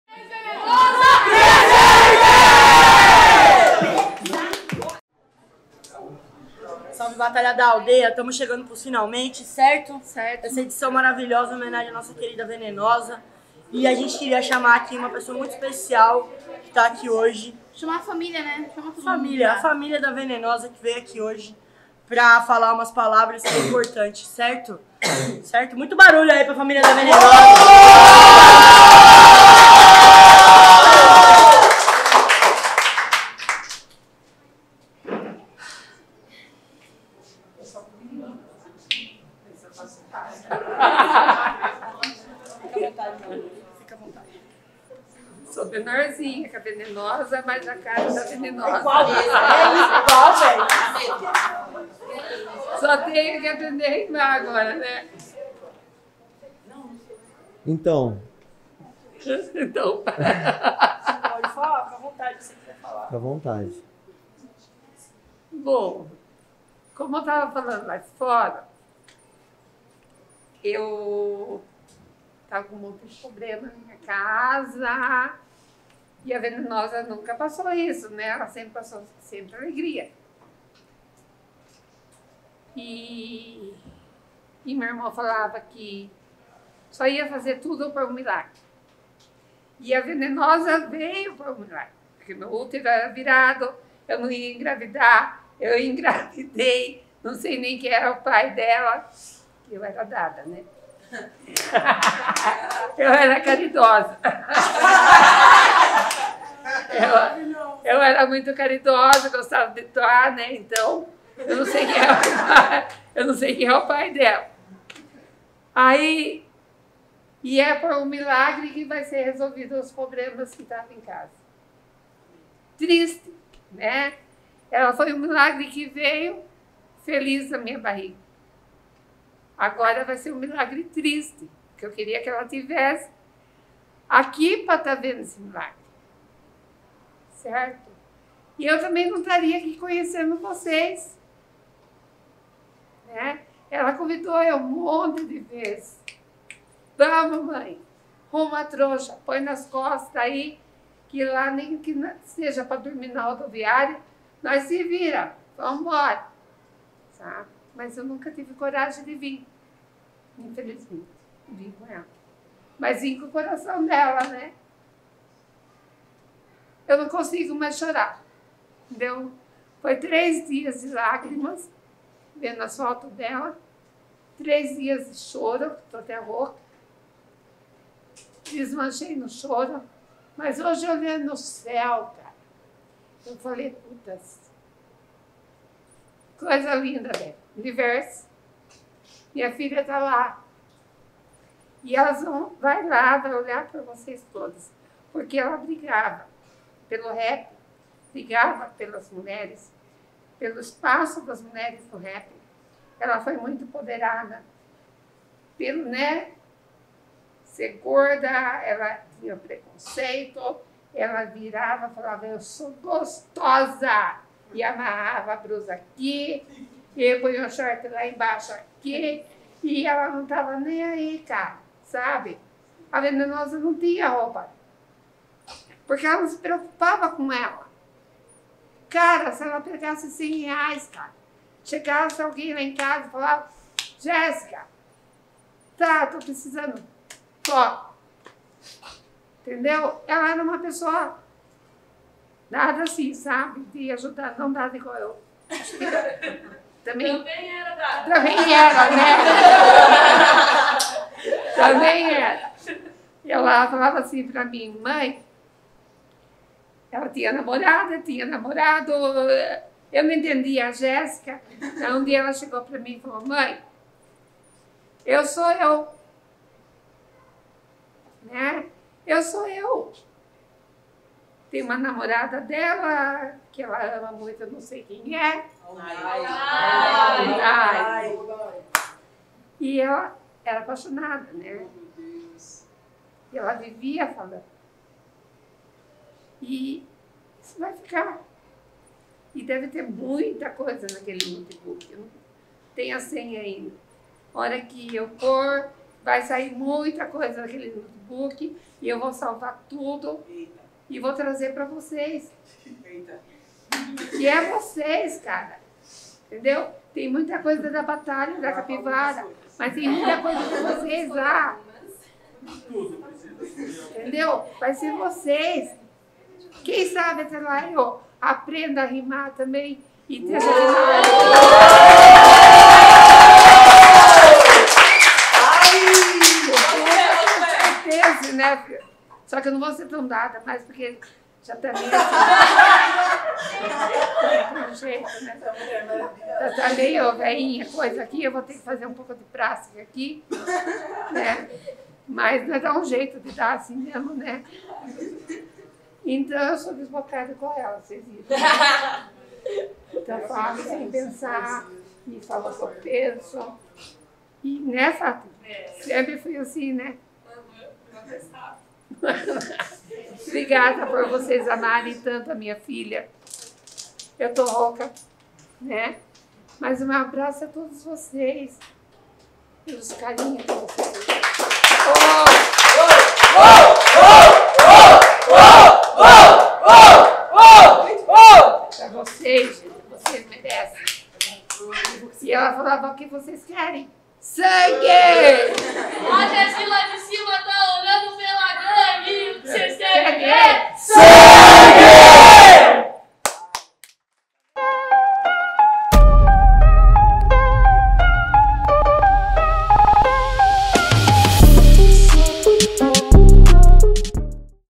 MAE DA VENENOSA EMOCIONA NA BDA | Batalha das Venenosas
A Batalha da Aldeia realizou junto a Batalha do Líbano um confronto de 8 MCs representantes de cada batalha sendo eles
BDA
Big Mike - @Bigao__
Noventa - @noventamc1
Tavin - oficial.tavin
refel - refelmc

Libano
Youngui -  @younguimc
Lincon - @lincoln_original
Balota - @balota.mc
Vinny - @vinnynine

Nessa semana perdemos a Venenosa, integrante muito importante do coletivo Batalha da Aldeia, Venenosa era apresentadora, MC e artista independente que batalhava dia apos dia pelo seu sonho de viver de rap, desejamos paz e tranquilidade a familia que precisa de ajuda nesse momento tão dificil. Se voce tem condições de ajudar a familia doe qualquer quantia na Vakinha a seguir: